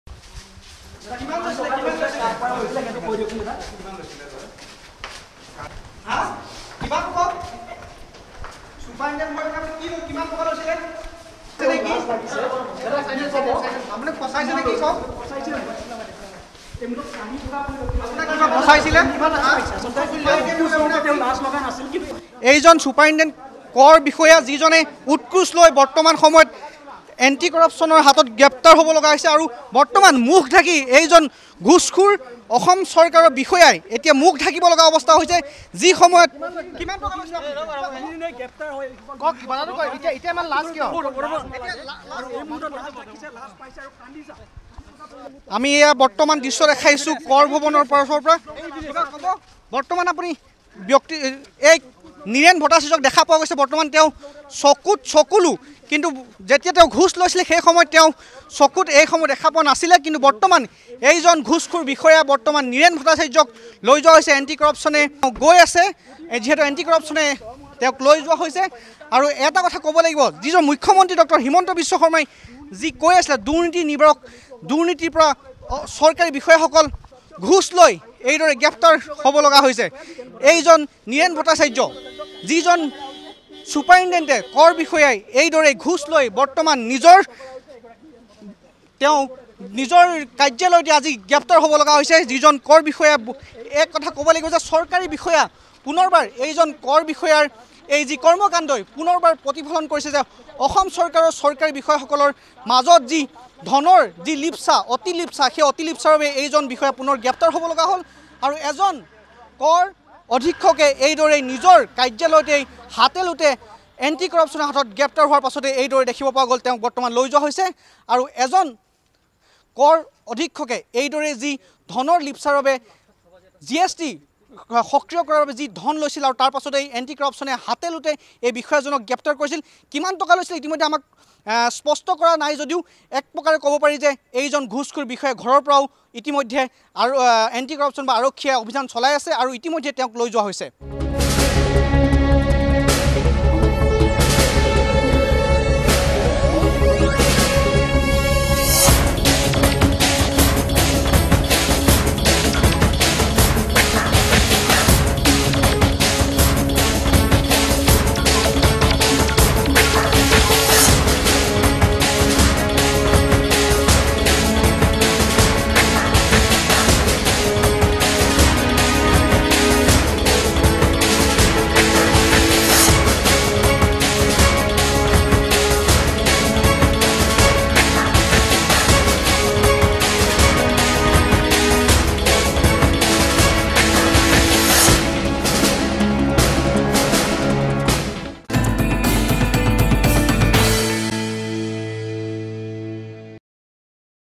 উৎকোচ লৈ হাতে-লোটে ধৰা পৰিল কৰ পৰিদৰ্শক নিৰেন ভট্টাচাৰ্য
🔴 উৎকোচ লৈ হাতে-লোটে ধৰা পৰিল কৰ পৰিদৰ্শক নিৰেন ভট্টাচাৰ্য। গুৱাহাটীৰ কৰ ভৱনত দুৰ্নীতি নিবাৰক শাখাই হাতে-লোটে  গ্ৰেপ্তাৰ কৰে পৰিদৰ্শক নিৰেন ভট্টাচাৰ্যক। ভট্টাচাৰ্যৰ হাতীগাঁৱৰ শেৱালি পথৰ বাসগৃহতো অভিযান দুৰ্নীতি নিবাৰক শাখাৰ।